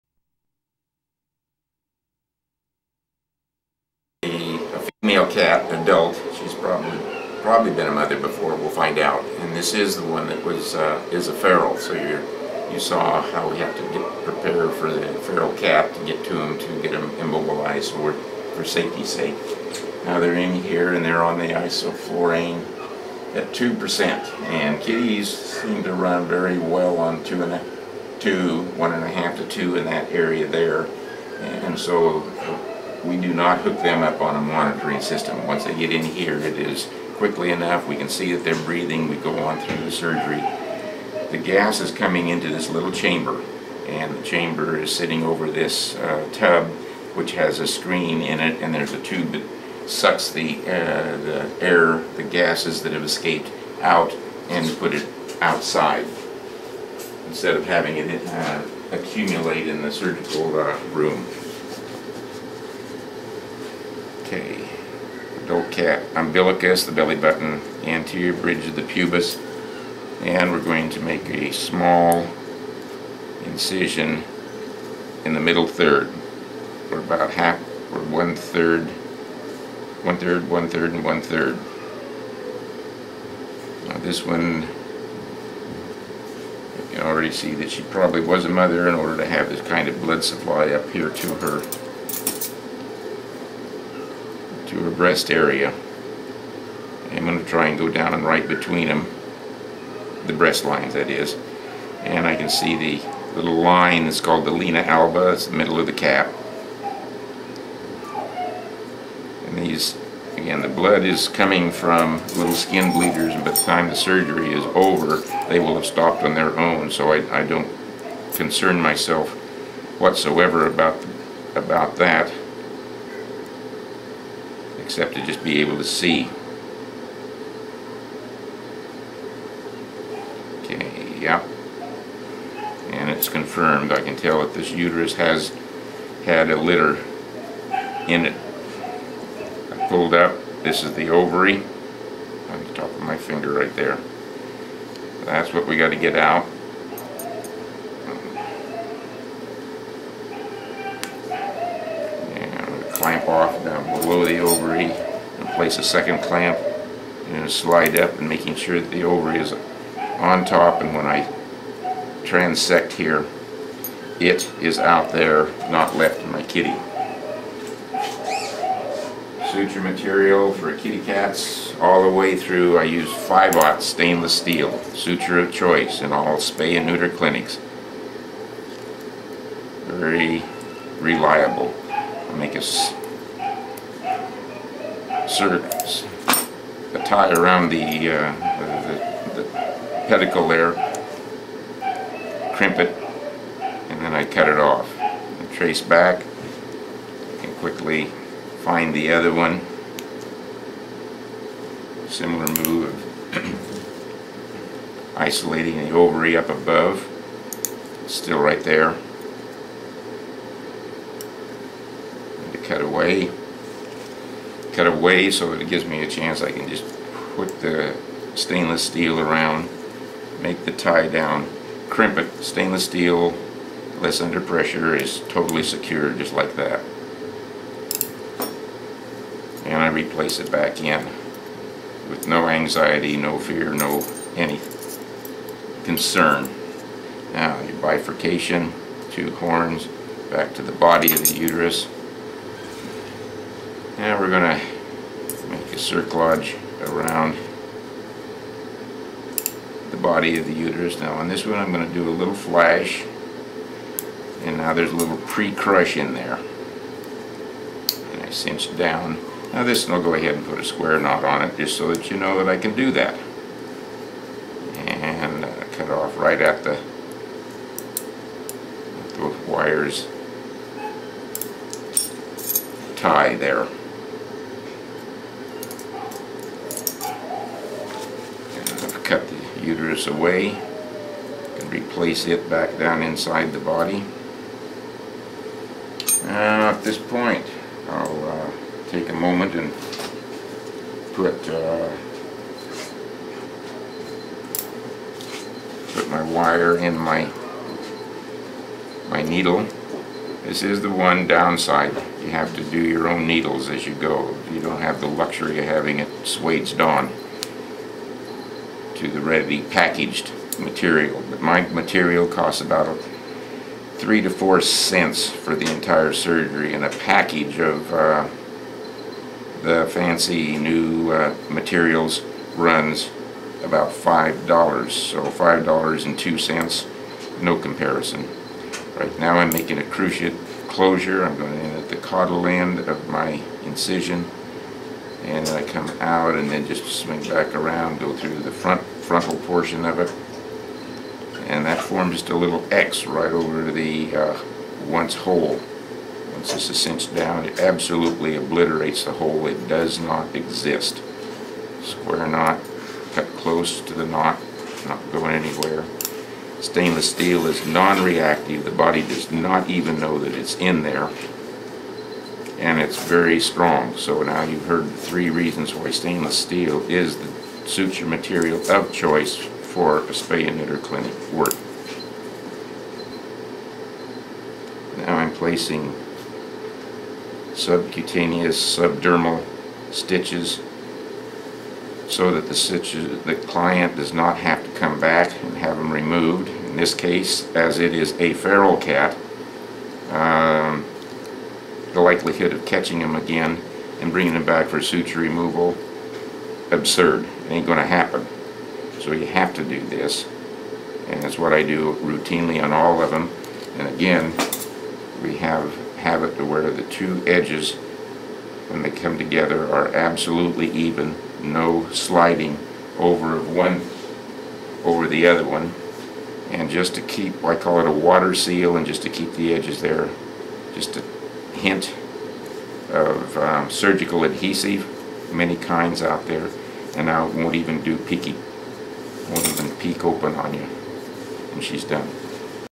Quickspay ~ Cat - Adult Female
Dr. W. Marvin Mackie of Animal Birth Control clinics in Southern California, demonstrates "QuickSpay", his high-volume surgical pet sterilization techniques developed while operating low-cost spay/neuter clinics since 1976.  This film shows the ease of surgery and rapid recovery of prepubescent clients.  Dr. Mackie has made personal appearances and taught hands-on training classes on early-age spay/neuter to veterinarians throughout the U.S., Europe, Japan, Australia, Mexico and Brazil for adaptation to their personal practices. and his film has been used in universities all over the world.